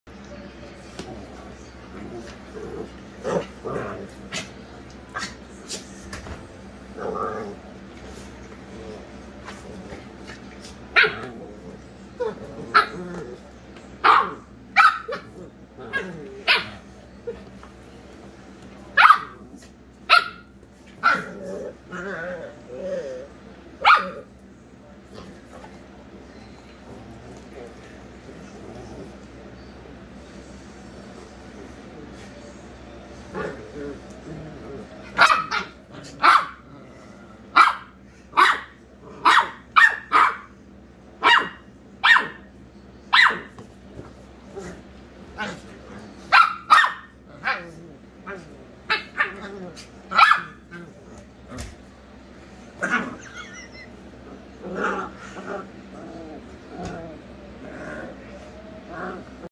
Penelope VonFluffyPants and Kona Kahuna having a fun lil argument
Our Corgi Penelope VonFluffyPants and our Dachshund Kona Kahuna were having a playful argument about who gets the bed